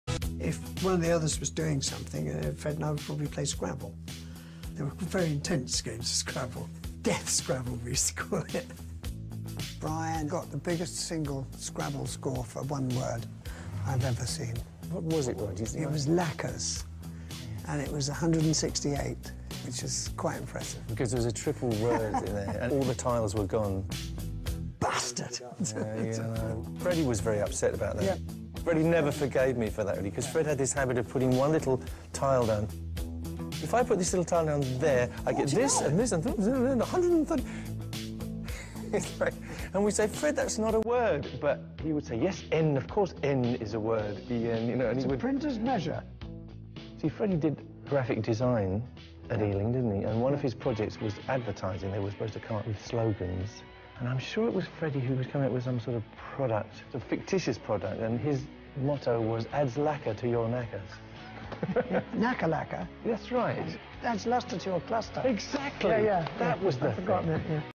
DeathScrabble
Queen Members reminiscing about old times.